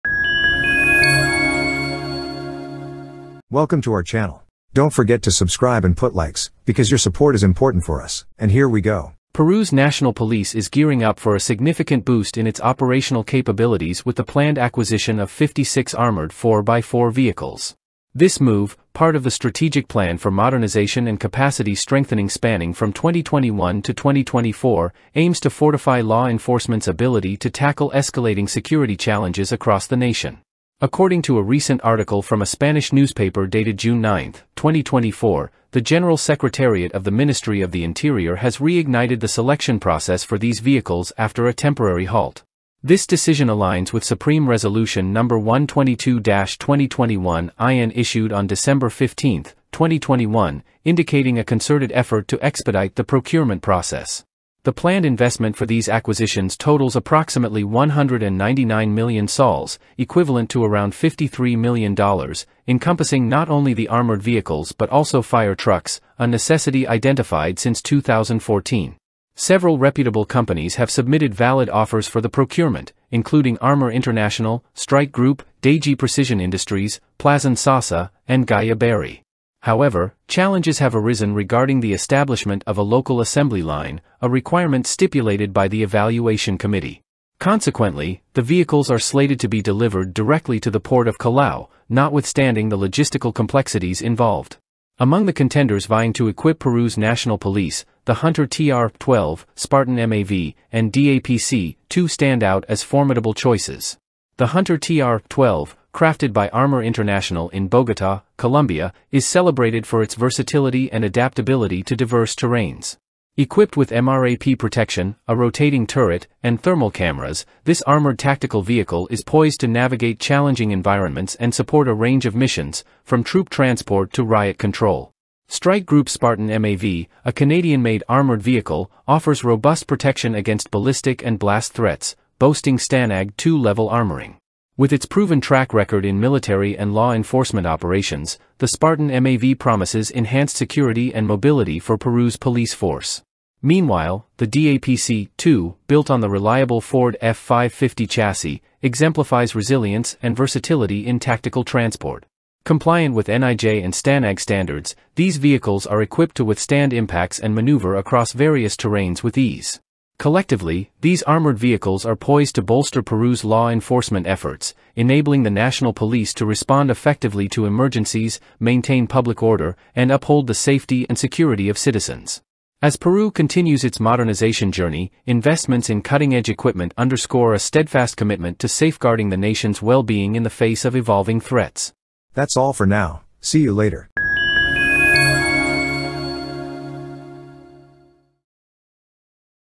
Peru Launches Tender to Strengthen Security Forces with 4x4 MRAPs
Peru's National Police is gearing up for a significant boost in its operational capabilities with the planned acquisition of 56 armored 4x4 vehicles. This move, part of the Strategic Plan for Modernization and Capacity Strengthening spanning from 2021 to 2024, aims to fortify law enforcement's ability to tackle escalating security challenges across the nation.